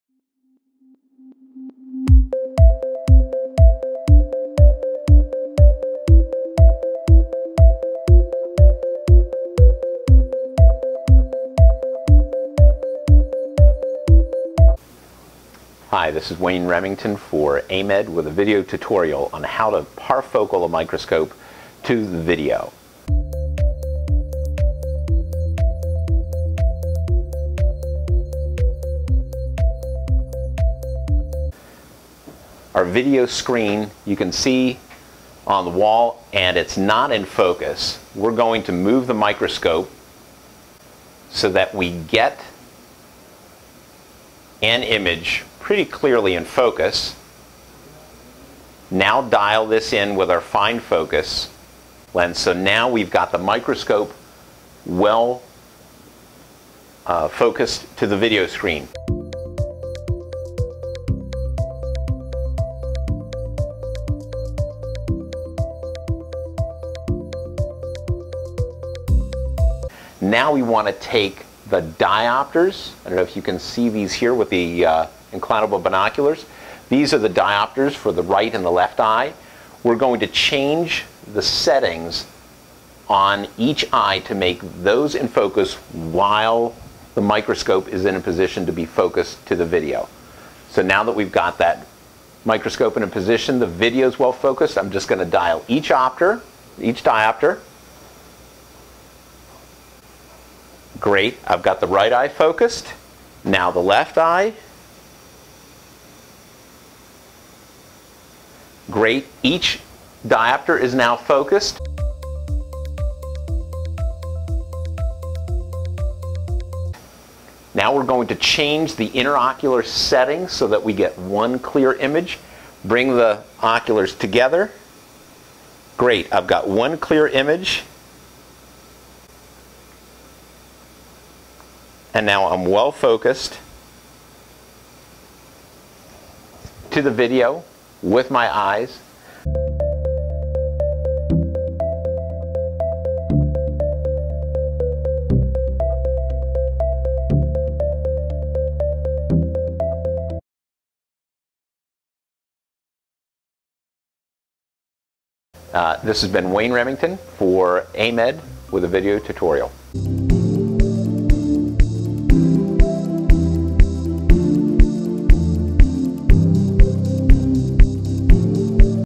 AMED Tutorial Parfocal to Video
AMED Tutorial Parfocal to Video.  Dr. Wayne Remington shows you step by step how to parfocal the microscope to video.  This step is necessary for completing the Certified Microscope User Level of AMED membership.